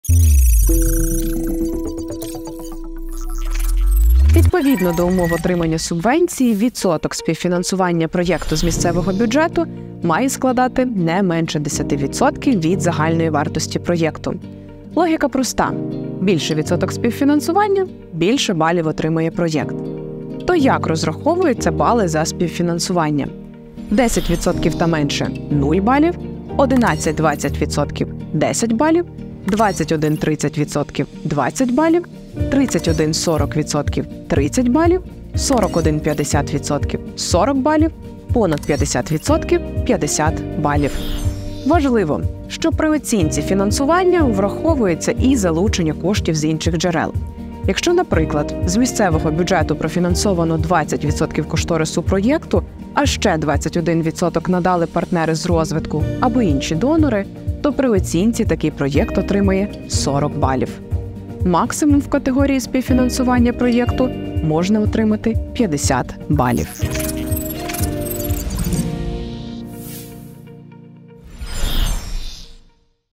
Співфінансування проєкту
Це показник, який визначає відсоток співфінансування проєкту з місцевого бюджету. Максимум в категорії можна отримати 50 балів.

Відеоролик створено за підтримки Дитячого фонду ООН (ЮНІСЕФ).